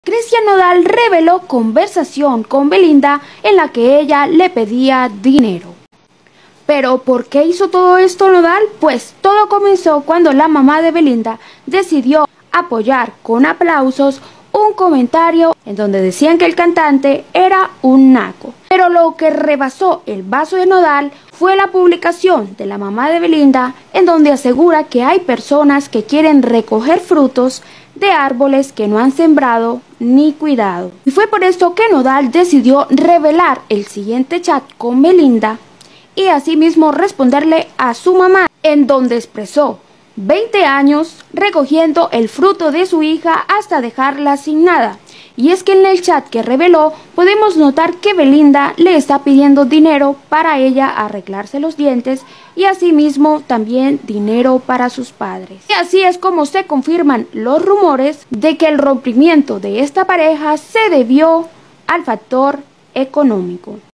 ¡Nodal rompió el silencio! Así se defendió de la mamá de Belinda
Cristian Nodal revela Chat de Belinda pidiéndole Dinero, todo esto por responderle a la mamá de Belinda